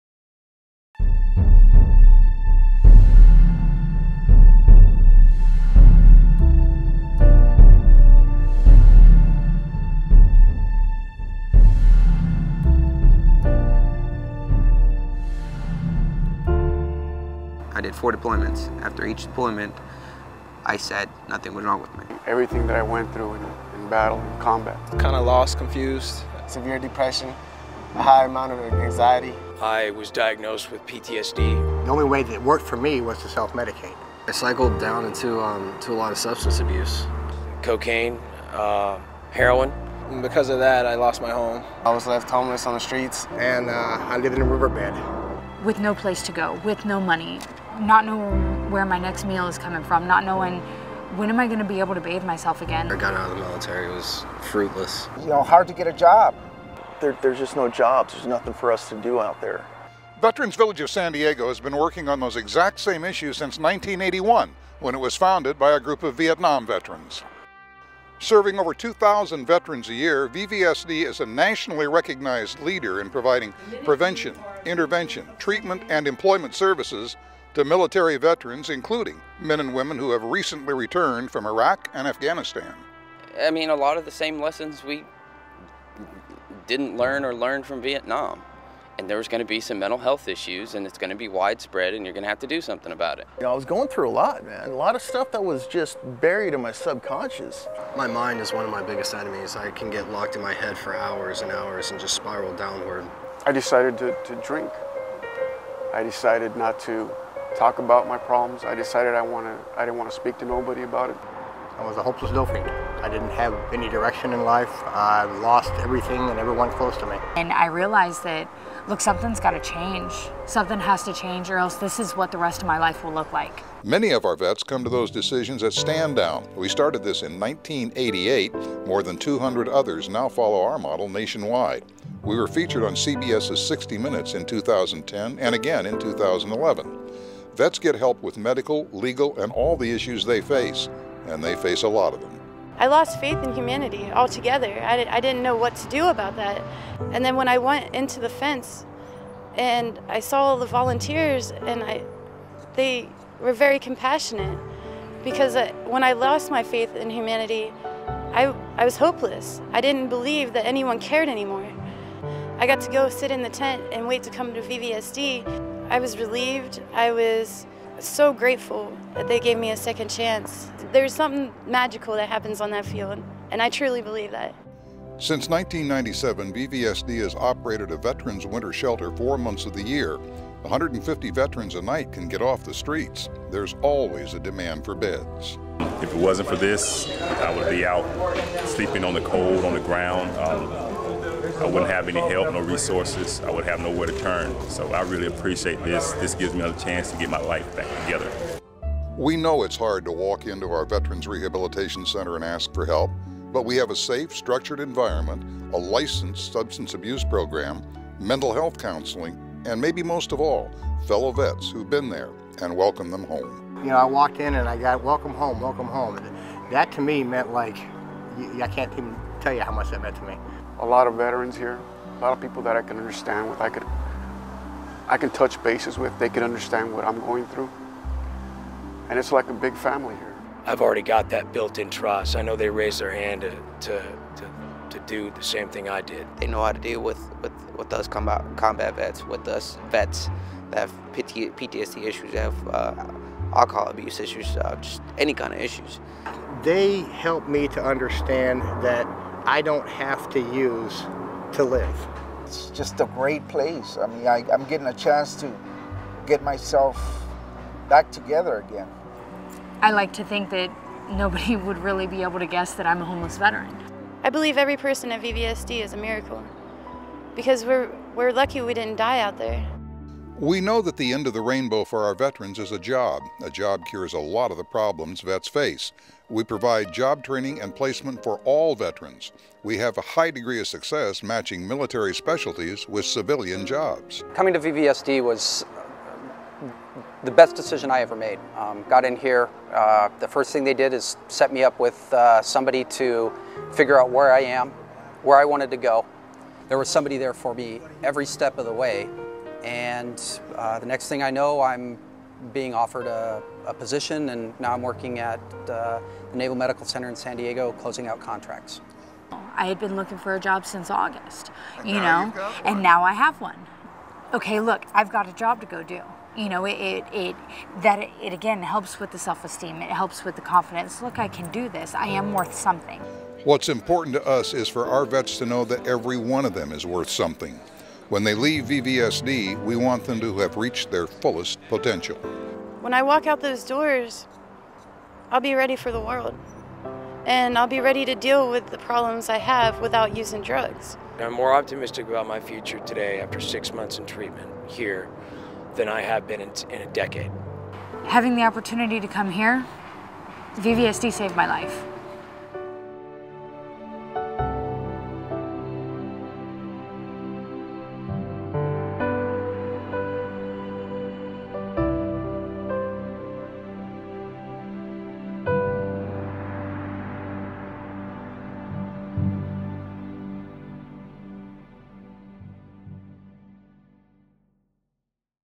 Veterans Village of San Diego Saved My Life
In the past 11 years, over 1.3 million American service personnel have served in Iraq and Afghanistan. Nearly 30% of them have returned home with traumatic brain injury, Post Traumatic Stress or both.
Some dealt with these issues by self medicating with drugs and alcohol. Most veterans cannot find work. Some have become homeless.

Since 2009, the number of Iraq and Afghanistan veterans assisted at VVSD has increased five-fold. And this is just the beginning. VVSD expects the number to increase many times over in the coming years.

In this emotional video some of the young veterans discuss their issues and how VVSD has assisted them